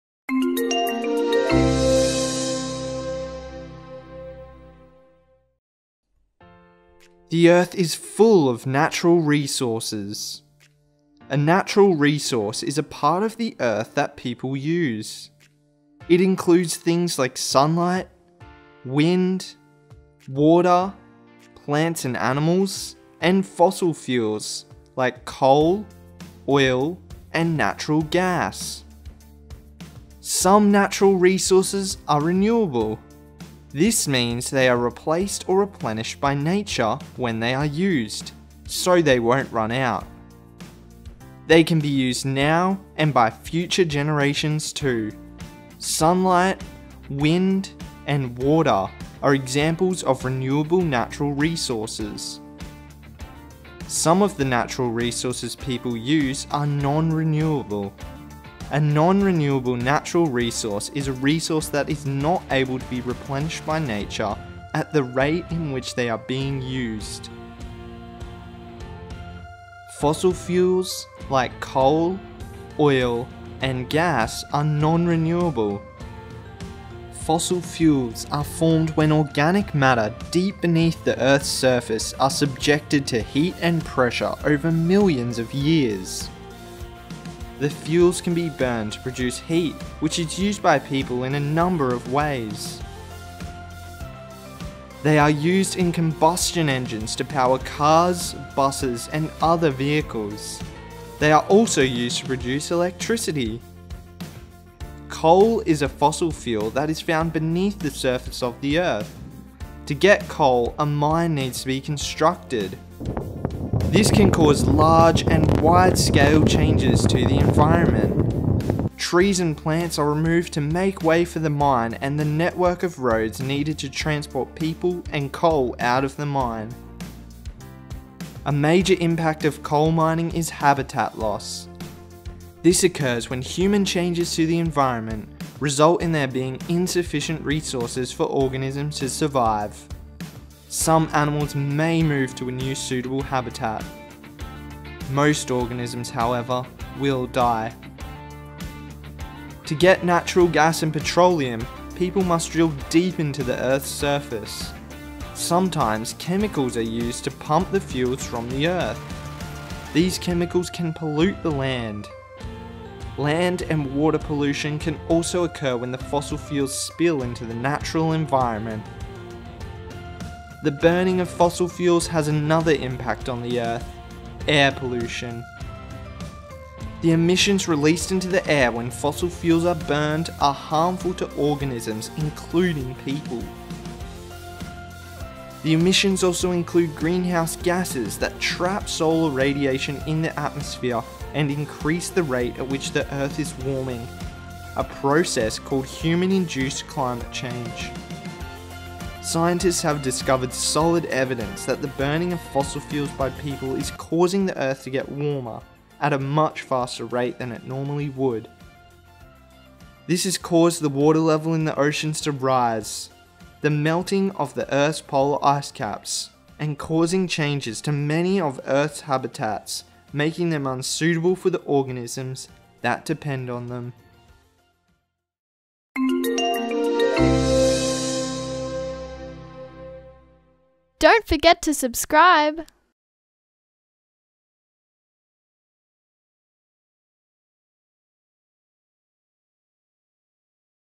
Renewable and Non-renewable Resources
All about the Earth's resources and how their use by people can impact the environment.

The Earth is full of natural resources. A natural resource is a part of the Earth that people use. It includes things like sunlight, wind, water, plants and animals and fossil fuels like coal, oil and natural gas.

Some natural resources are renewable. This means they are replaced or replenished by nature when they are used...so they wont run out. They can be used now and by future generations too. Sunlight, wind and water are examples of renewable natural resources.

Some of the natural resources people use are non-renewable. A non-renewable natural resource is a resource that is not able to be replenished by nature at the rate in which they are being used. Fossil fuels, like coal, oil and gas are non-renewable. Fossil fuels are formed when organic matter deep beneath the Earth's surface are subject to heat and pressure over millions of years. The fuels can be burned to produce heat which is used by people in a number of ways. They are used in combustion engines to power cars, buses and other vehicles. They are also used to produce electricity.

Coal is a fossil fuel that is found beneath the surface of the Earth. To get coal, a mine needs to be constructed. This can cause large and widescale changes to the environment. Trees and plants are removed to make way for the mine and the network of roads needed to transport people and coal out of the area. A major impact of coal mining is habitat loss. This occurs when human changes to the environment result in there being insufficient resources for organisms to survive. Some animals may move to a new suitable habitat. Most organisms however, will die.

To get natural gas and petroleum, people must drill deep into the Earth’s surface. Sometimes chemicals are used to pump the fuels from the Earth. These chemicals can pollute the land. Land and water pollution can also occur when these fossil fuels spill into the natural environment.

The burning of fossil fuels has another impact on the Earth - air pollution. The emissions released into the air when fossil fuels are burned are harmful to organisms, including people. The emissions also include greenhouse gases that trap solar radiation in the atmosphere and increase the rate at which the Earth is warming - a process called human-induced climate change. Scientists have discovered solid evidence that the burning of fossil fuels by people is causing the Earth to get warmer at a much faster rate than it normally would. This is caused the water level in our oceans to rise, the melting of  the Earth's polar ice caps and causing changes to many of Earth's habitat making them unsuitable for the organisms that depend on them.